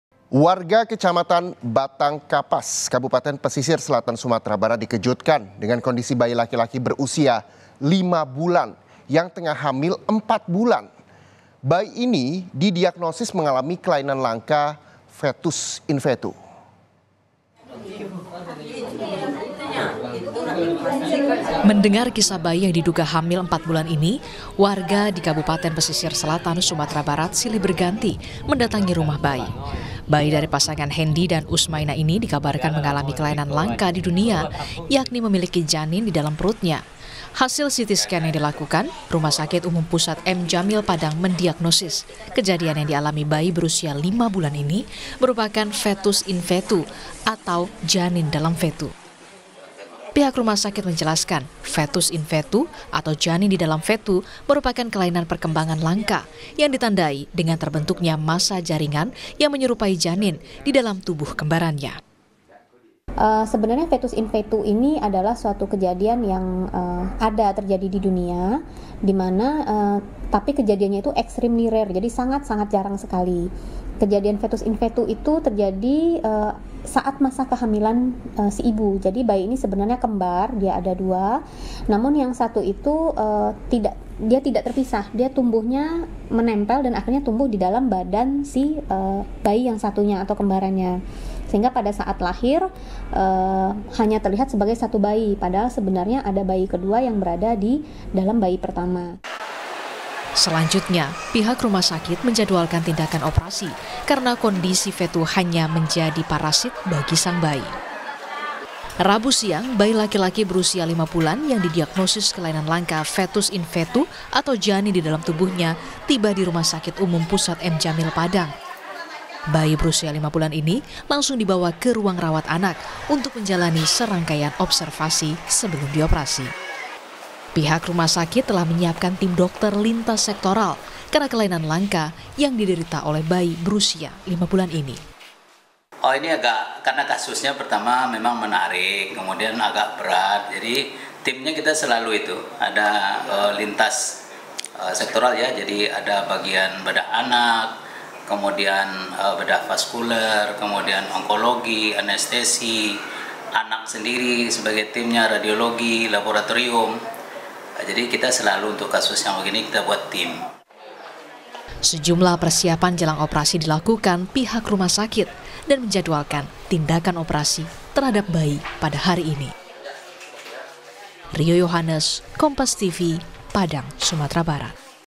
Viral Kasus Bayi Laki-Laki Hamil 4 Bulan di Sumbar Akan Jalani Operasi
SUMBAR, KOMPAS.TV - Warga Kecamatan Batang Kapas, Kabupaten Pesisir Selatan, Sumatera Barat dikejutkan dengan kondisi bayi laki-laki berusia 5 bulan yang tengah hamil 4 bulan.

Bayi ini didiagnosis mengalami kelainan langka fetus in fetu.

Pihak rumah sakit telah menyiapkan tim dokter lintas sektoral karena kelainan langka yang diderita oleh bayi berusia lima bulan ini.

Sejumlah perisapan jelang operasi dilakukan pihak rumah sakit dan menjadwalkan tindakan operasi terhadap bayi pada hari ini (26/10/23).